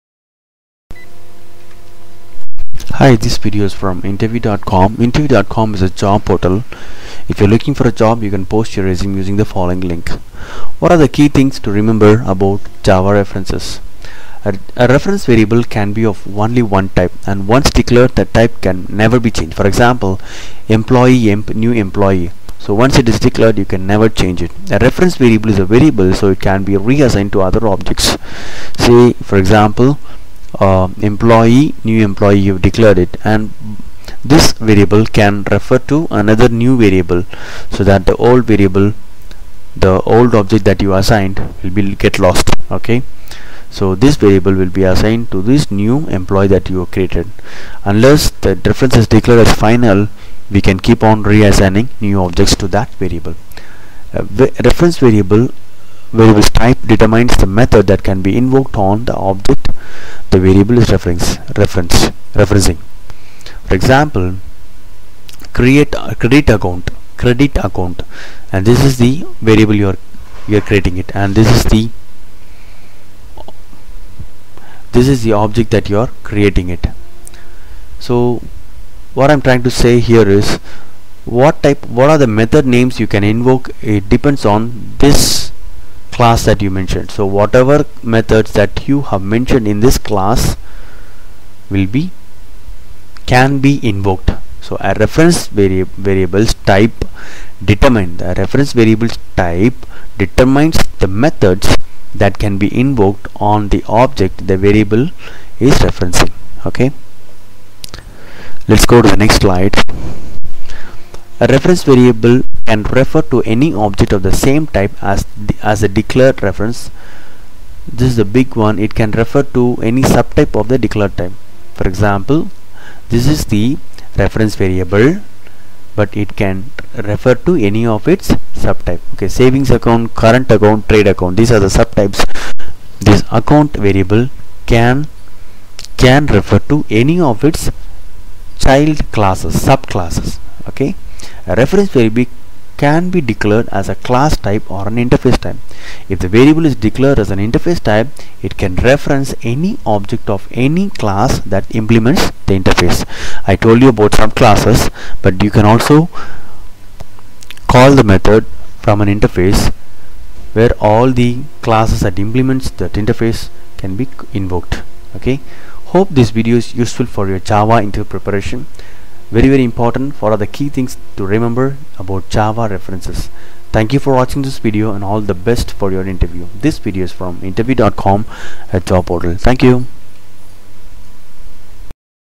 Java references key things explain java interview question interviewdot
to get notifications. Java references key things explain java interview question interviewdot
What are the key things to remember about java references ?

  A reference variable can be of only one type, and once declared, that type
  can never be changed.

 Employe emp = new Employee();

  A reference is a variable, so it can be reassigned to other objects, (unless the
 reference is declared final).
 emp = new Employee();

  A reference variable's type determines the methods that can be invoked on
the object the variable is referencing.
 CreditAccount creAct = new CreditAccount();
 creAct.credit(fromAcc, toAcc);
A reference variable can refer to any object of the same type as the declared
 reference, or—this is the big one—it can refer to any subtype of the
 declared type.

Account  SavingsAccount
                  CurrentAccount
                 TradeAccount

Account acc = new CurrentAccount();

  A reference variable can be declared as a class type or an interface type. If
 the variable is declared as an interface type, it can reference any object of any
class that implements the interface.